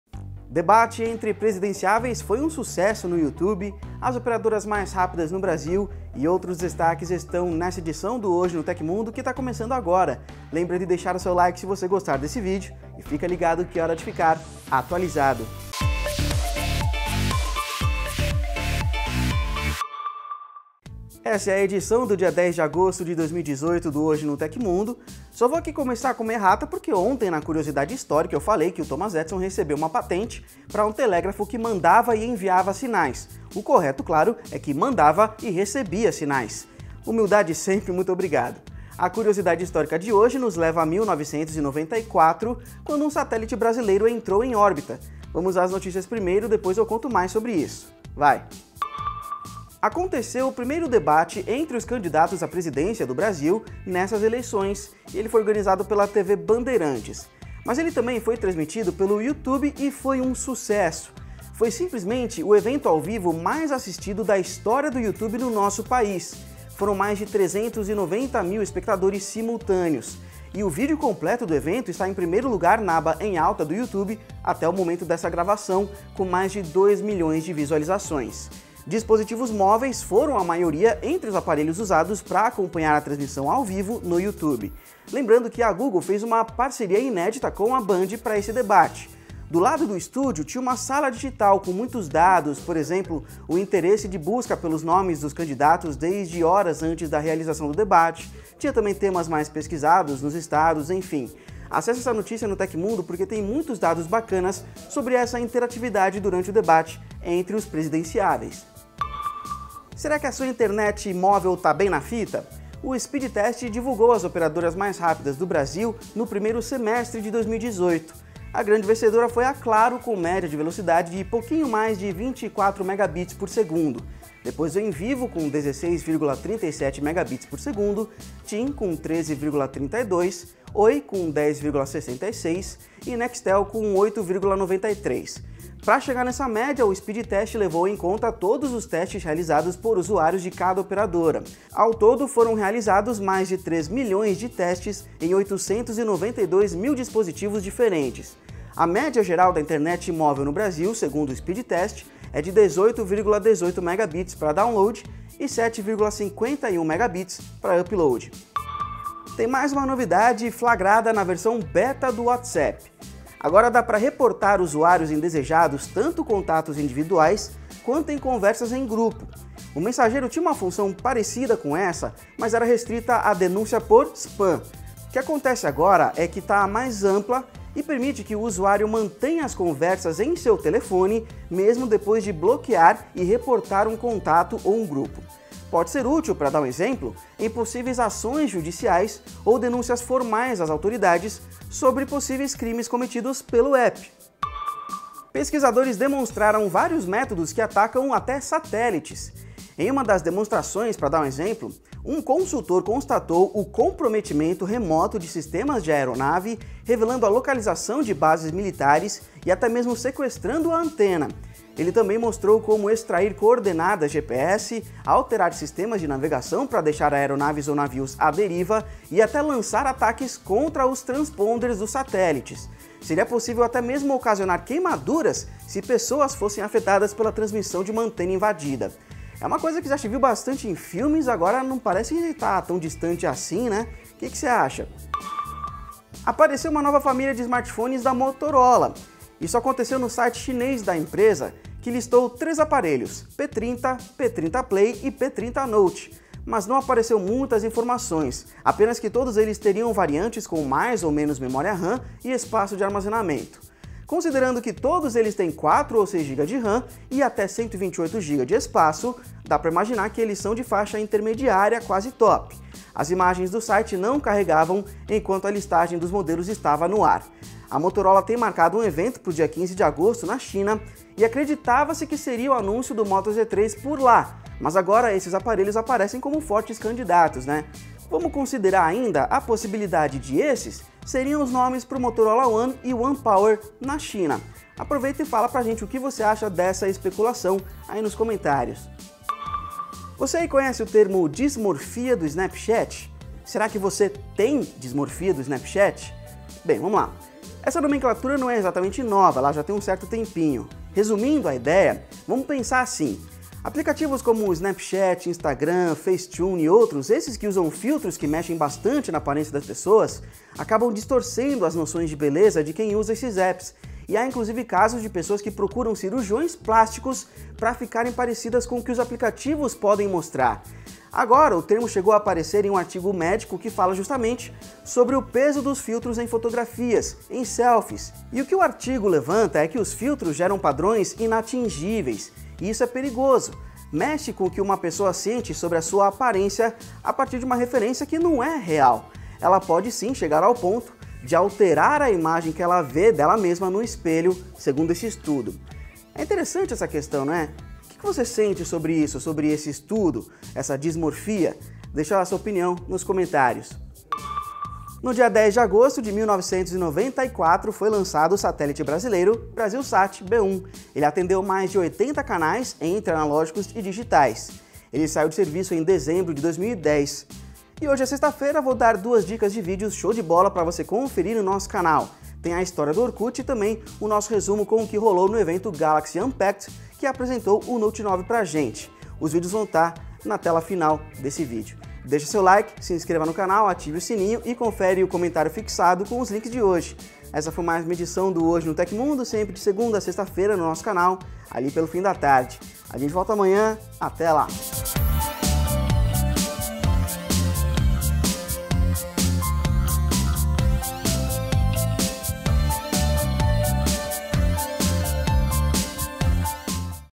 Debate entre presidenciáveis no YouTube, Dismorfia do Snapchat e mais - Hoje no TecMundo
Links e tempos das notícias estão no comentário fixado.

CRÉDITOS
Texto: Danilo Amoroso
Apresentação: Danilo Amoroso
Imagens: Rodrigo Doze Vana
Edição de vídeo: Rodrigo Doze Vana
Arte da thumb: Silmara Slobodzian